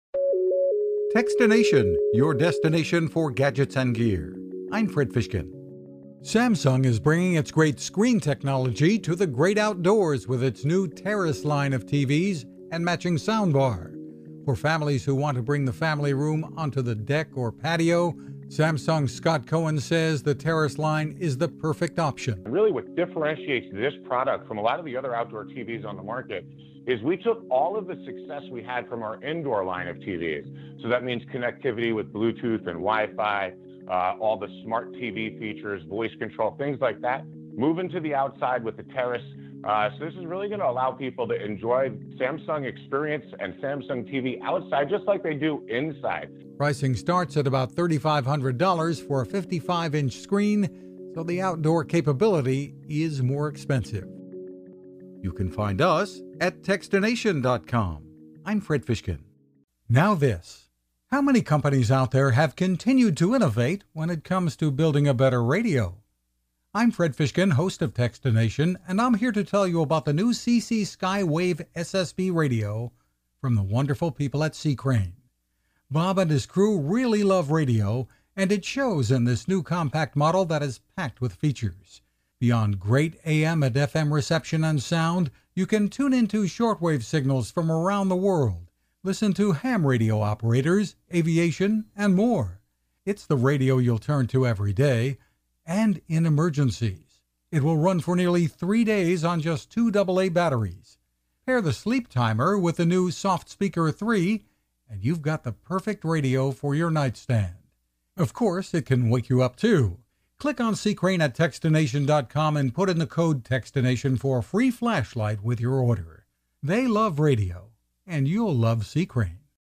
Samsung delivers outdoor "Terrace" screens & more
Techstination, your destination for gadgets and gear.  I’m Fred Fishkin.  Samsung is bringing its great screen technology to the great outdoors with it’s new Terrace line of TVs and matching soundbar.  For families who want to bring the family room onto the deck or patio…Samsung’s Scott Cohen says…the Terrace line is the perfect option…

“And really what differentiates this product from a lot of the other outdoor TVs that are on the market is we took all of the success we had from our indoor line of TVs  …so that means connectivity with Bluetooth and WiFi …all the smart TV features..voice control, things like that..move into the outside with the Terrace.  So this is really going to people to enjoy Samsung experience and Samsung TV just like they do inside.”

Pricing starts at about 35 hundred dollars for a 55 inch screen…so the outdoor capability is more expensive.